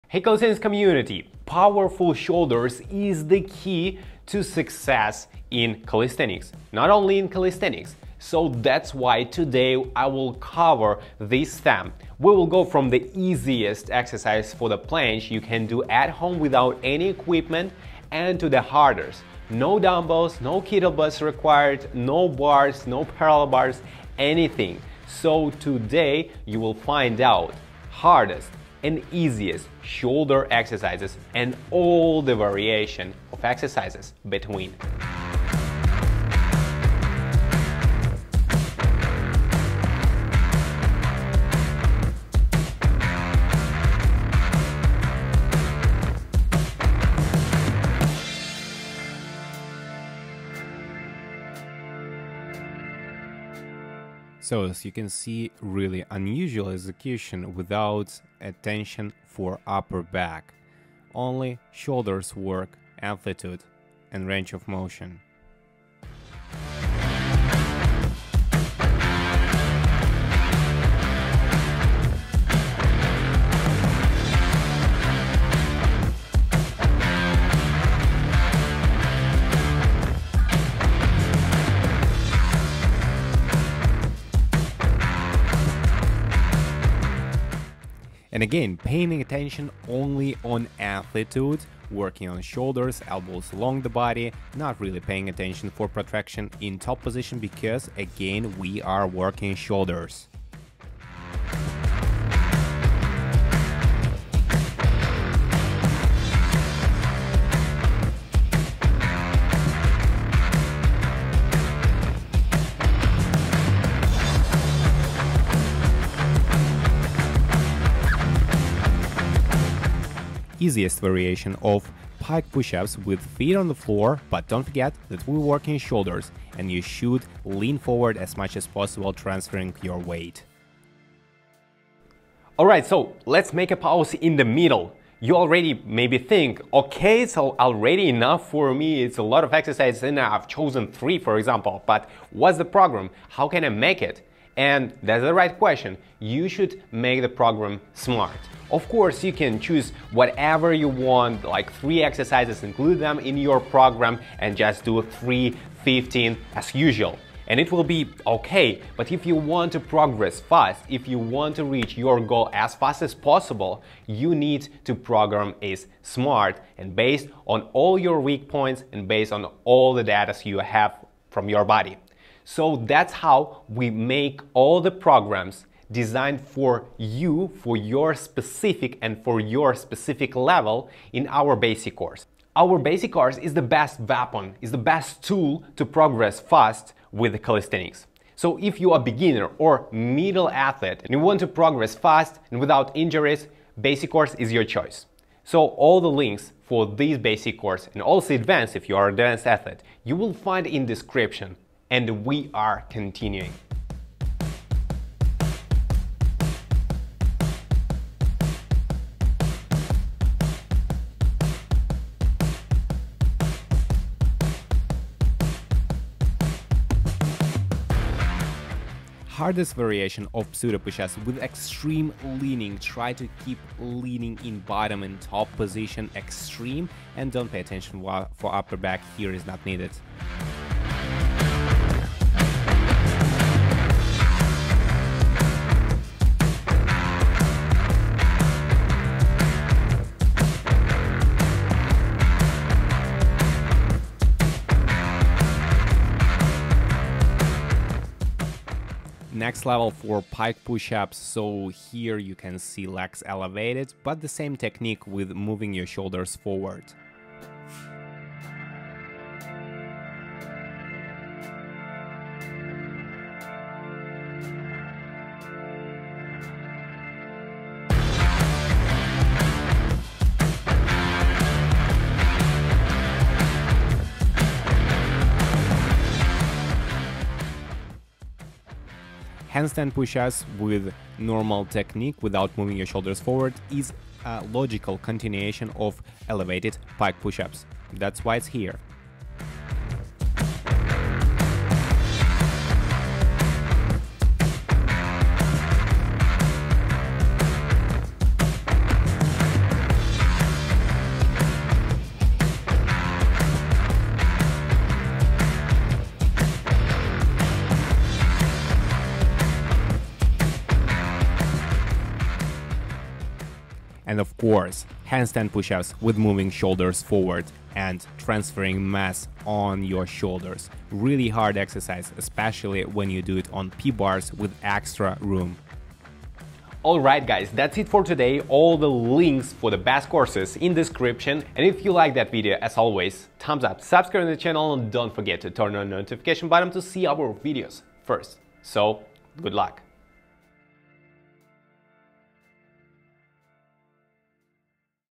The PERFECT Shoulder Exercises (NO WEIGHTS). FOR ALL LEVELS.
Today we will take a look at the best Shoulder Exercises (NO WEIGHTS). You can do this exercises at home with no equipment. Will be useful for basic preparation for planche.

PLANCHE 1000% COURSE YOU WILL FIND HERE